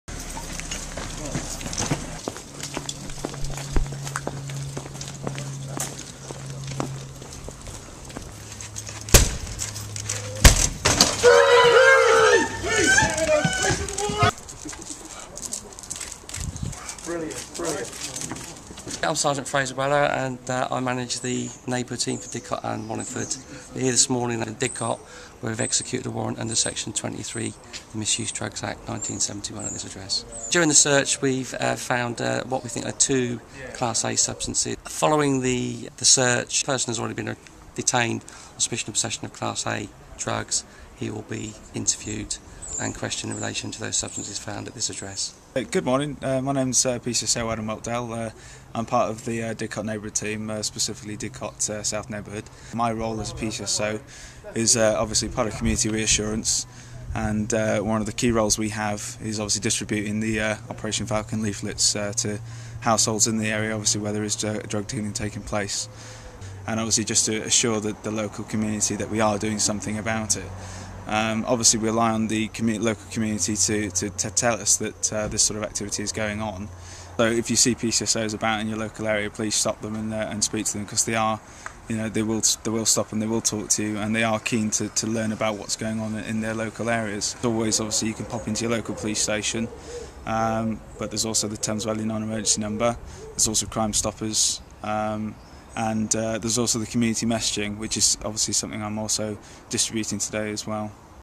Drugs raid in Didcot
The Didcot Neighbourhood Policing team executed a drugs warrant in Didcot today (21/7).

They arrested a 27-year-old man on suspicion of possession of Class A drugs with intent to supply.

The team rely on information from their local community - if you have any information or concerns, please contact the Thames Valley Police non-emergency number on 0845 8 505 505.